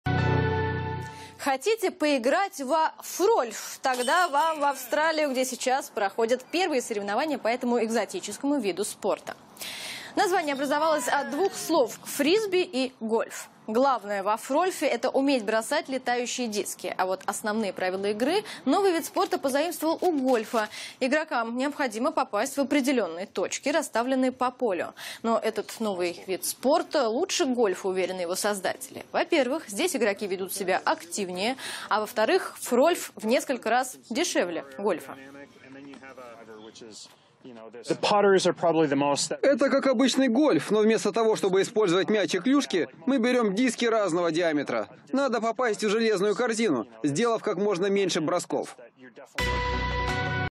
В Австралии проходят первые соревнования по фрольфу
В Австралии изобрели новый вид спорта - фрольф